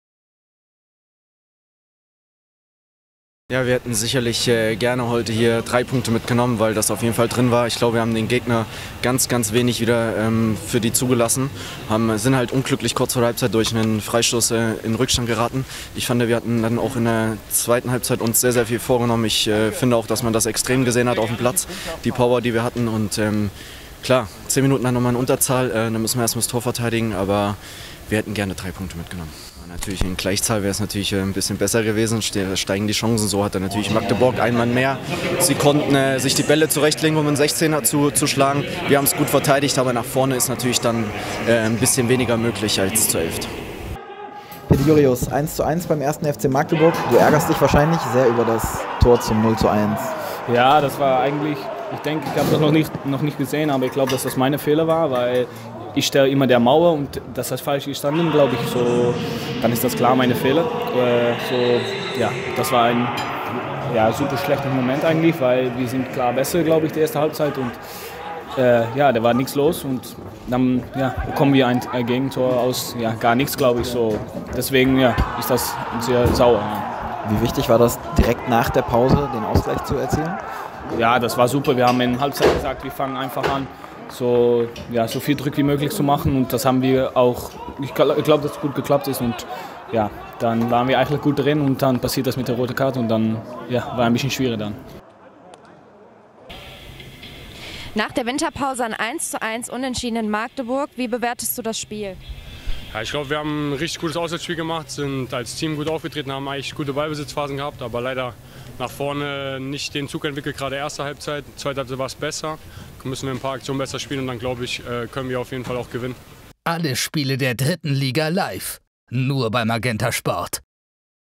KFC.tv: Stimmen & Tore vom 1:1 in Magdeburg
Nach dem 1:1 beim 1. FC Magdeburg haben wir für euch die Kommentare von Hidde Jurjus, Patrick Göbel und Tim Albutat sowie die beiden Treffer des Spiels.